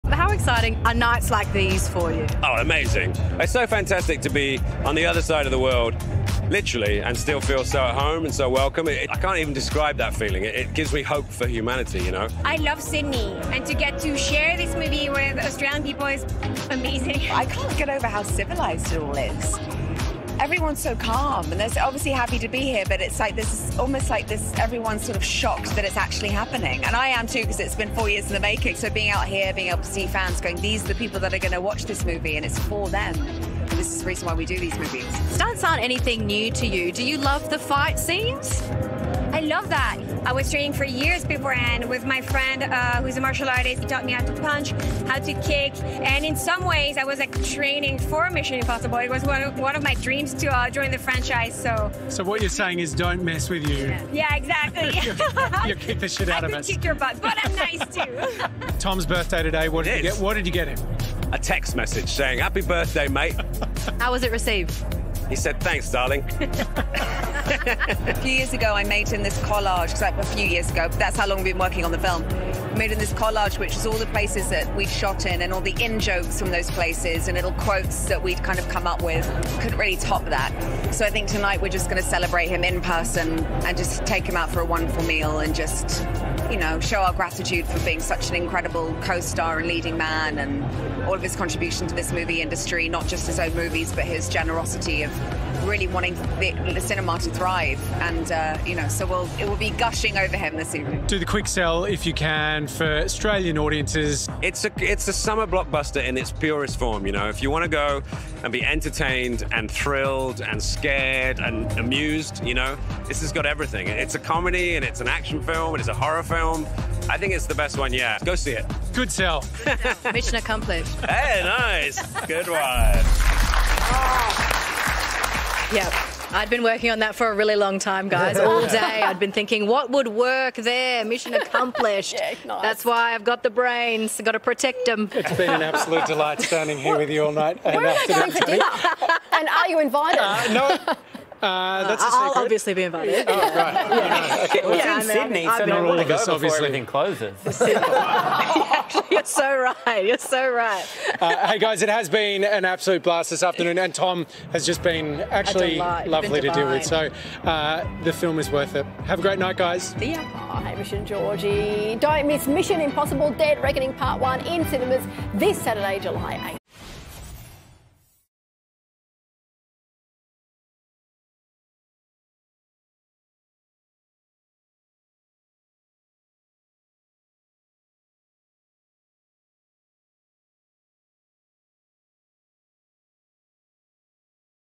Mission: Impossible Cast On What They Got Tom Cruise For His Birthday
It's a big day for the Mission Impossible cast at the Sydney red carpet premiere... and it's Tom Cruise's birthday! So we asked Simon Pegg, Hayley Atwell and Pom Klementieff what they got one of the biggest names in Hollywood.